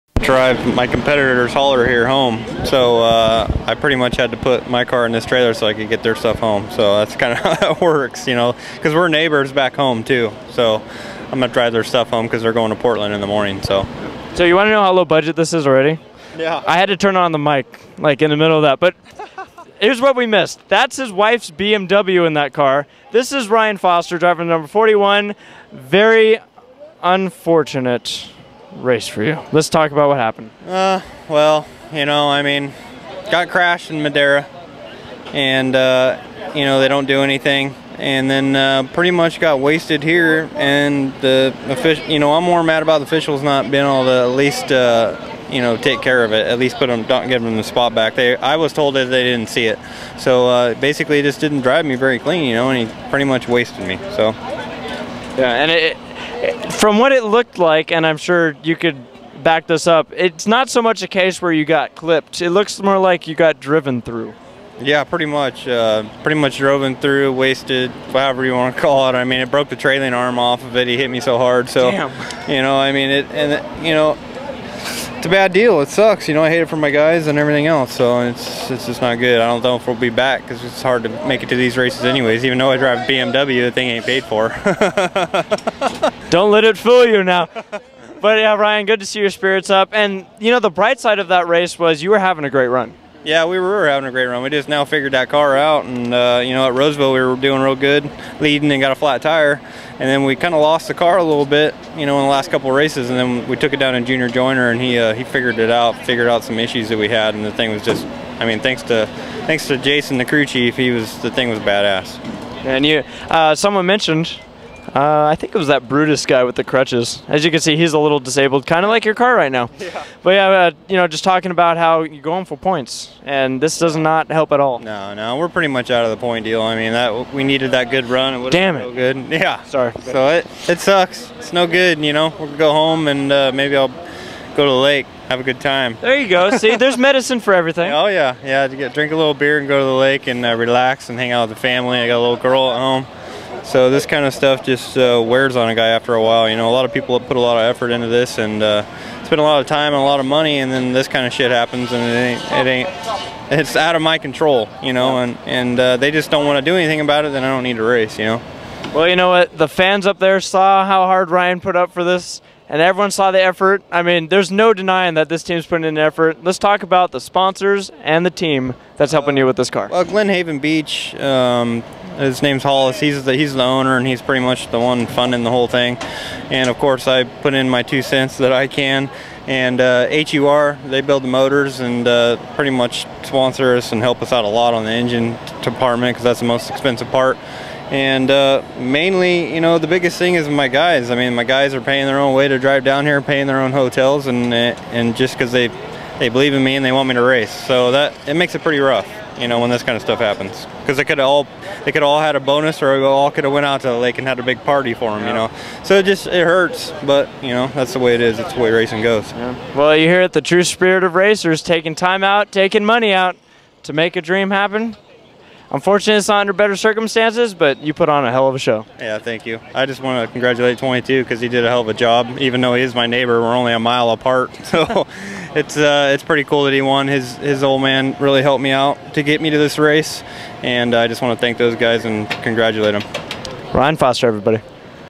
Ryan Foster SRL 7-17
Toyota Speedway at Irwindale SRL July 17, 2010 Ryan Foster #41 .  Running great, got taken out...here's his side of the story.